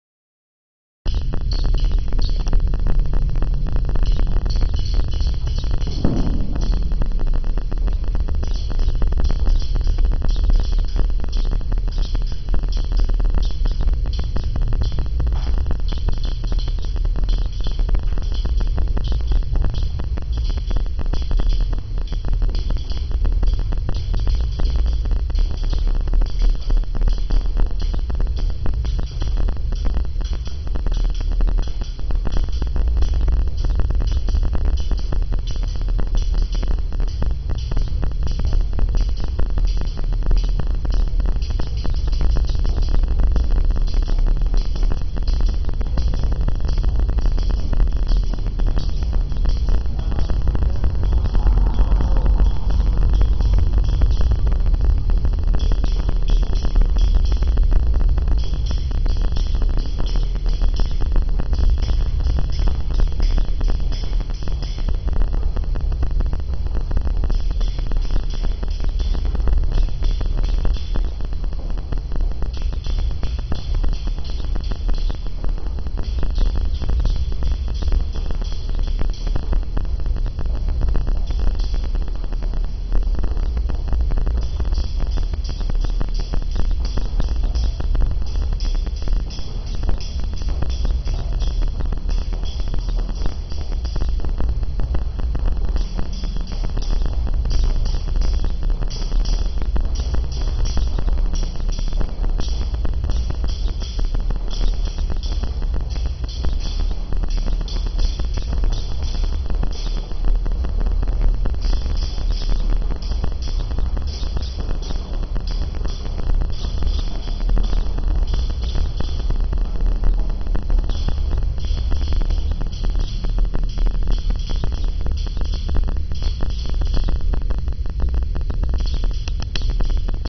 singuhr hoergalerie
a quick tour through the sound installation by Paul Desmarinis "around the word / the probably flight path of AF447" at the grosser wasserspeicher in Prenzlauerberg, Berlin